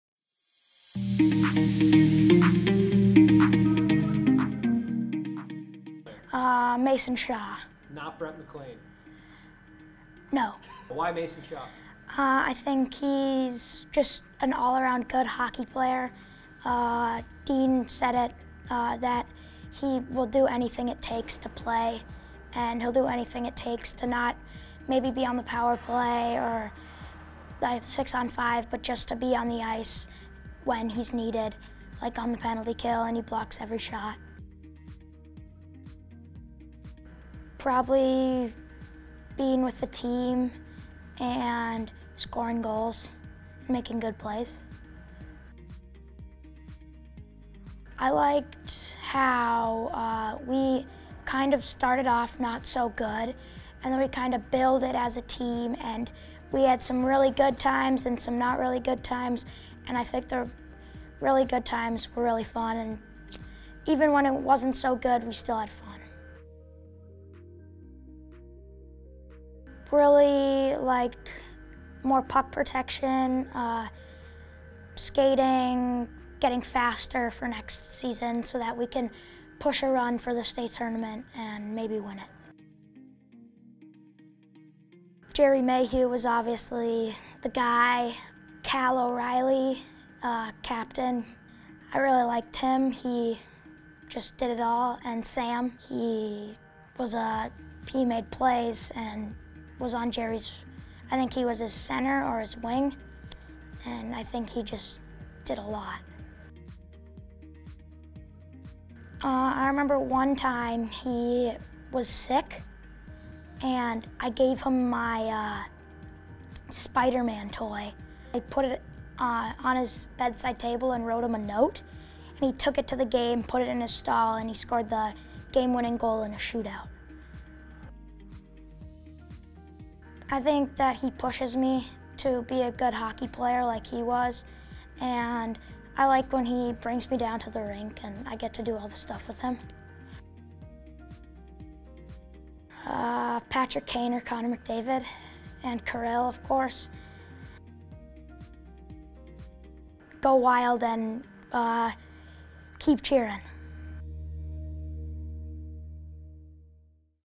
Future Faces | Nixon McLean
It's our last episode of Future Faces before the guys hit the ice in Iowa next week... but you may be waiting a lot longer to see him playing in an Iowa Wild sweater. Meet Nixon McLean, son of head coach Brett McLean! Hear a bit about Nixon's hockey career and what it's like to have Coach McLean as a dad.